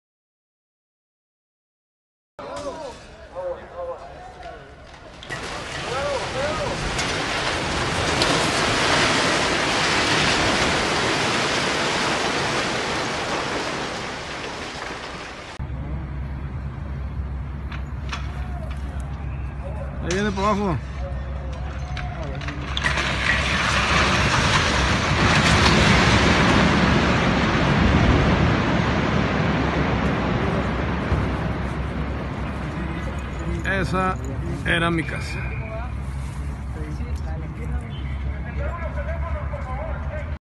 Momento exacto del derrumbe de una casa en Monterrey
El mediodía de este miércoles se produjo el derrumbe de tres viviendas en un predio de la colonia Alfareros en Monterrey, que causó la muerte de una persona y una más lesionada. Aquí se observa el colapso de una de ellas desde varios ángulos.